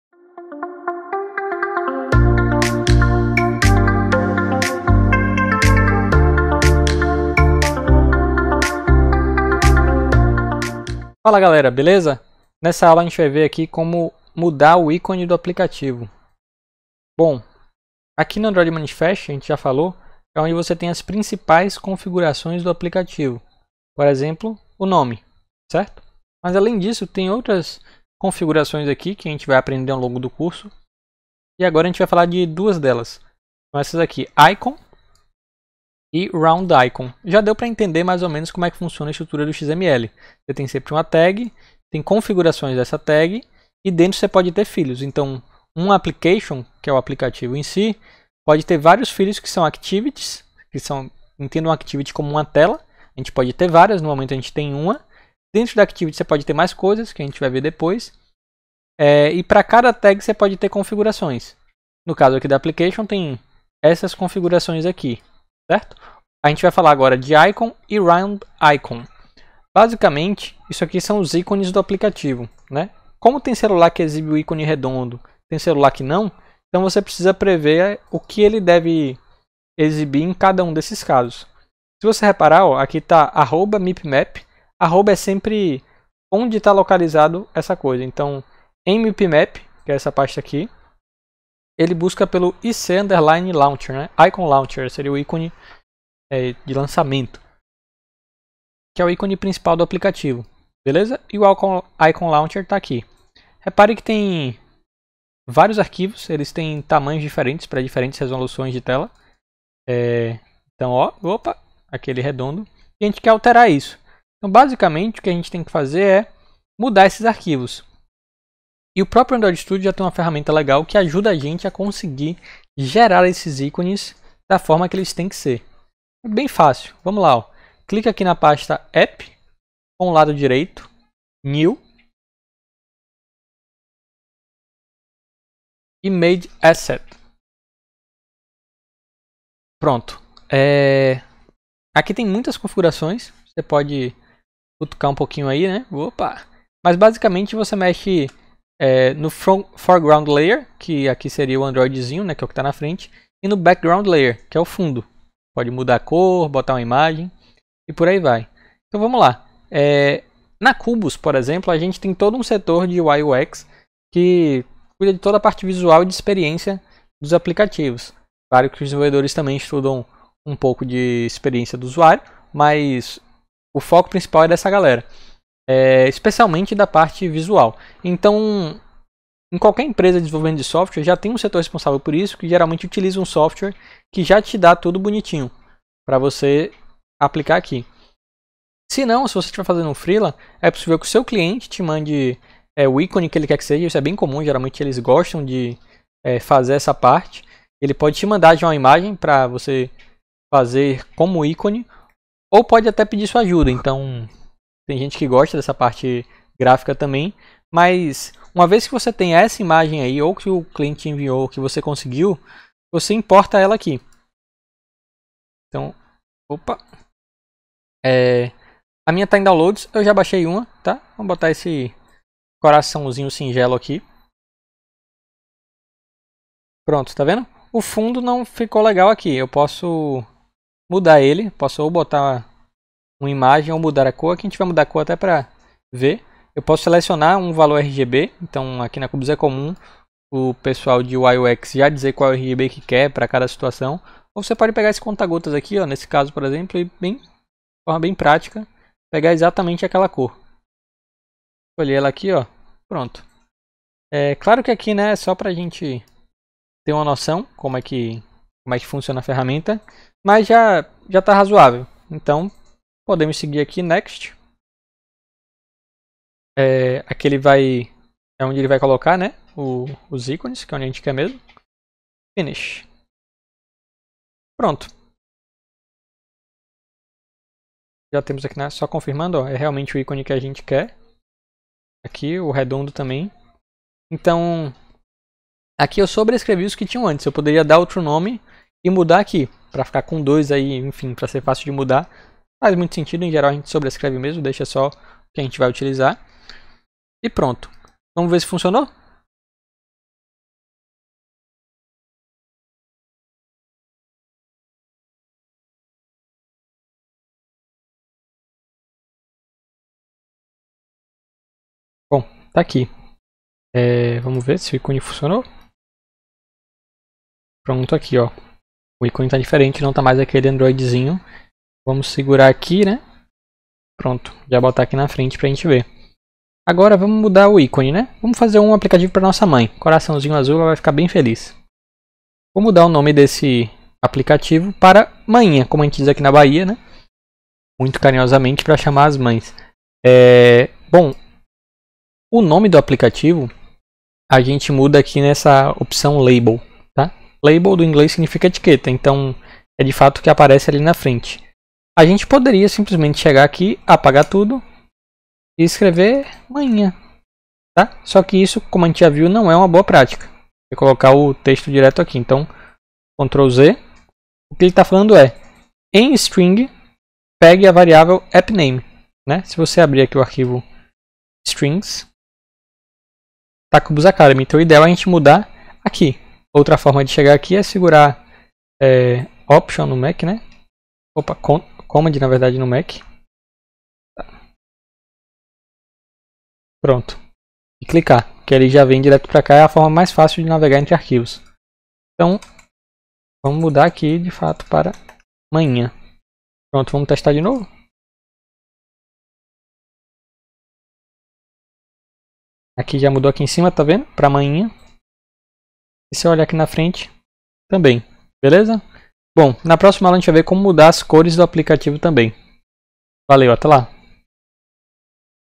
MÓDULO 03: Aula 02 - Como mudar o ícone do app | Cubos Academy
Aprenda a criar seu próprio aplicativo Android do zero com esse curso gratuito fornecido pela Cubos Academy. No módulo 3 você vai aprender a personalizar o seu aplicativo, como mudar as cores, ícones e muito mais!